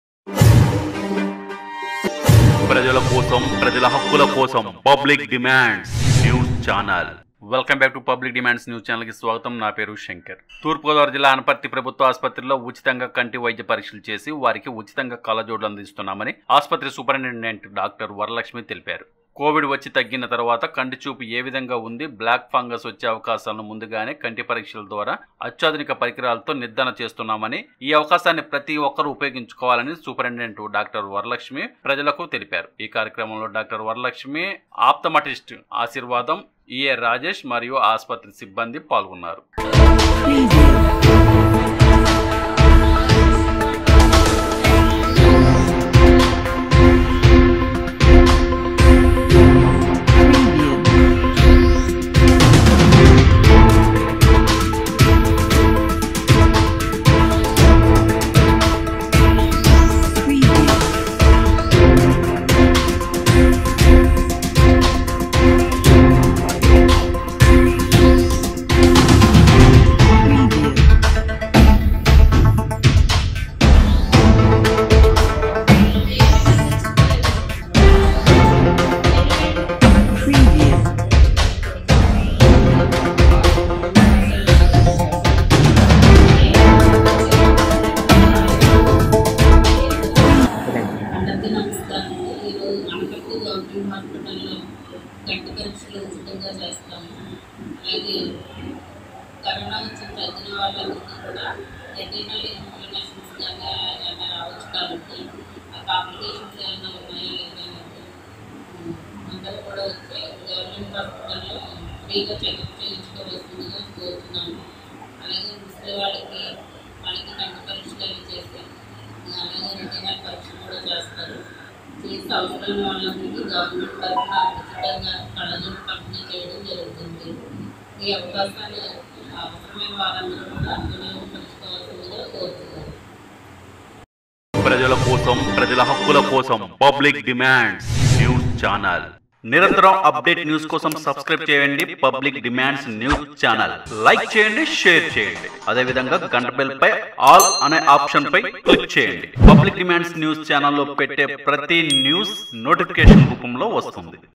తూ.గో.జిల్లా, అనపర్తి ప్రభుత్వాసుపత్రిలో ఉచితంగా కంటి వైద్య పరీక్షలు PUBLIC DEMANDS NEWS
తూ.గో.జిల్లా, అనపర్తి ప్రభుత్వాసుపత్రిలో ఉచితంగా కంటి వైద్య పరీక్షలు చేసి, వారికి ఉచితంగా కళ్ళజోడ్లు అందజేస్తున్నామని ఆస్పత్రి సుపరిటెండెంట్ డాక్టర్ వరలక్ష్మి తెలియజేసారు, అత్యాధునిక పరికరాలతో కోవిడ్ వచ్చి తగ్గిన తరువాత కంటిచూపు ఏవిధంగా ఉంది, బ్లాక్ ఫంగస్ వచ్చే అవకాశాలను ముందుగానే కంటి పరీక్షల ద్వారా నిర్దారణ చేస్తున్నామని, ఈ అవకాశాన్ని ప్రతీ ఒక్కరూ ఉపయోగించుకోవాలని సుపరిటెండెంట్ డాక్టర్ వరలక్ష్మి ప్రజలను తెలిపారు, ఈ కార్యక్రమంలో డాక్టర్ వరలక్ష్మి, ఆప్థమటిస్ట్ ఆశీర్వాదం, EA రాజేష్ మరియు ఆస్పత్రి సిబ్బంది పాల్గొన్నారు

     ꧂Thankyou ꧂